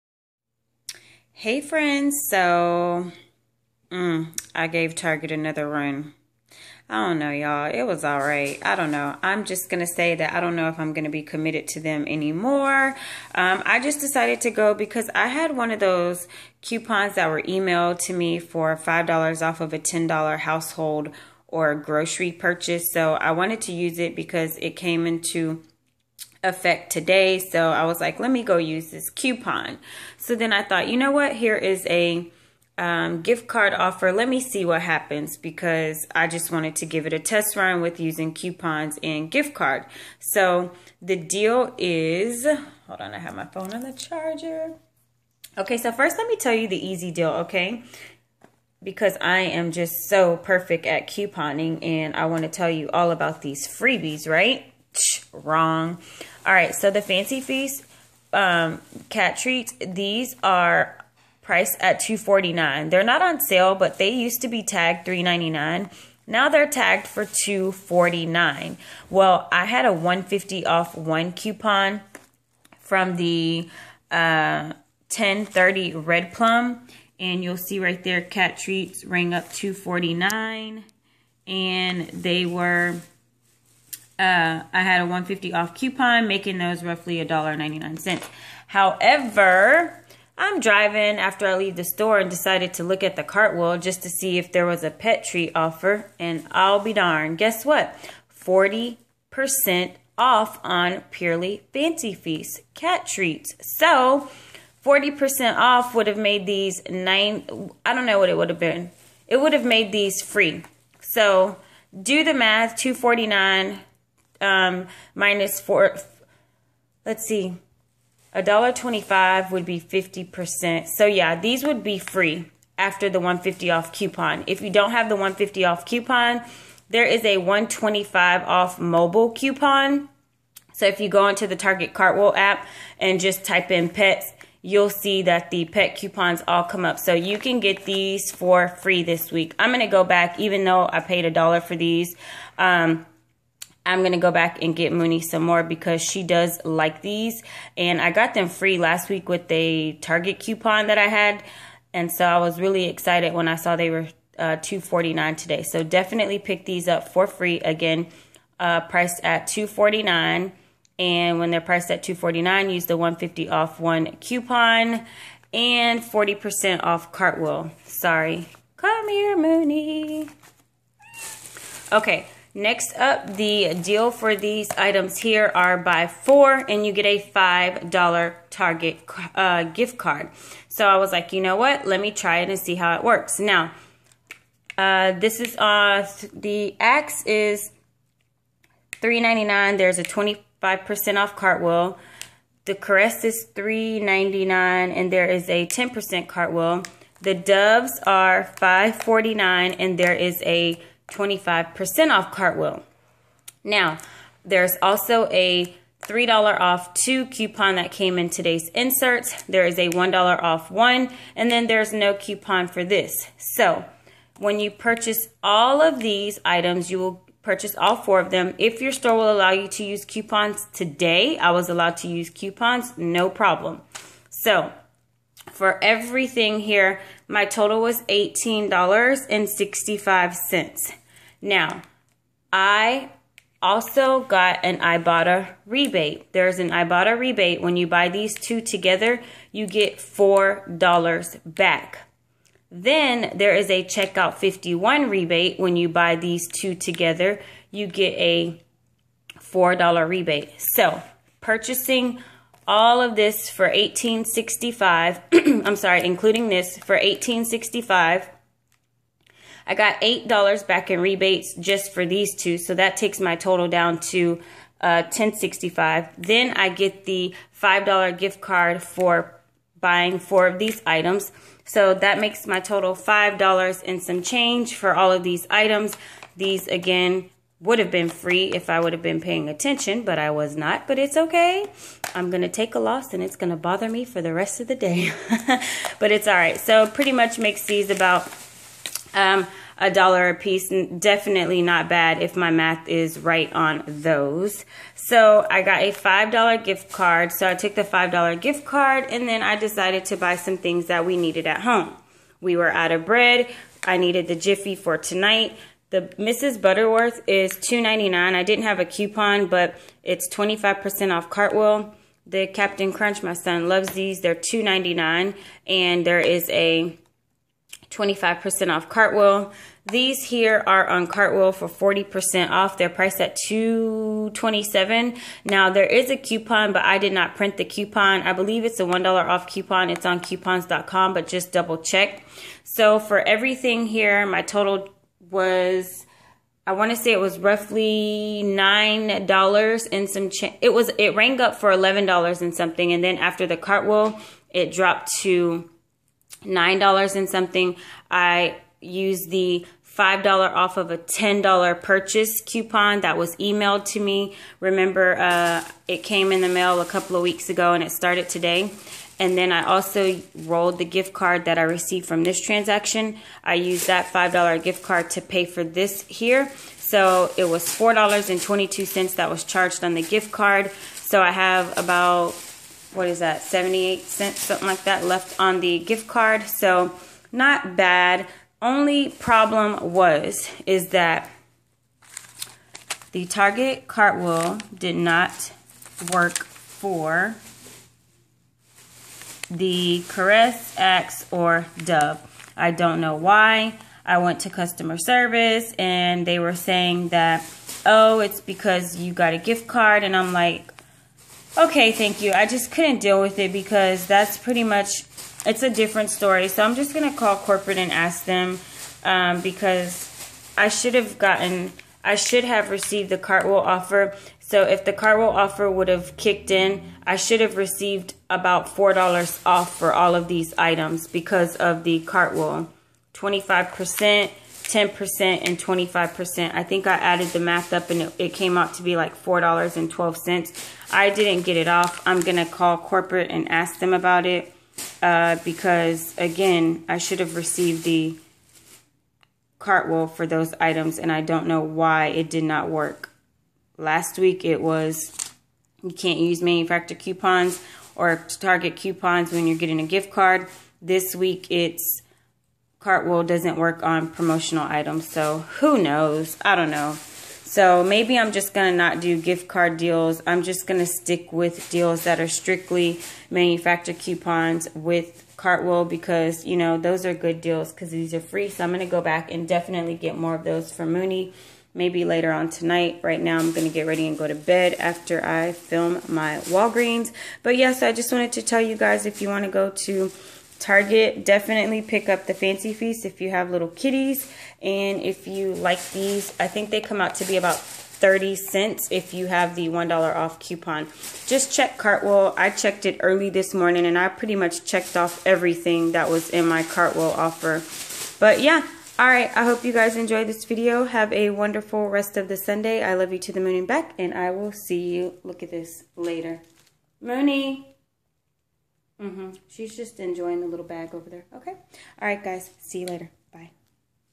Target Couponing 11/6 | FREE Cat Treats & CHEAP CHEAP BODYWASH
Happy Sunday!!! Don't forget Ibotta $4 Rebate back & Checkout 51 $4 Rebate back for Axe & Caress!

Ibotta: Join My Ibotta Team: Do you use Ibotta?

If you read all of this You're The Real MVP's!!!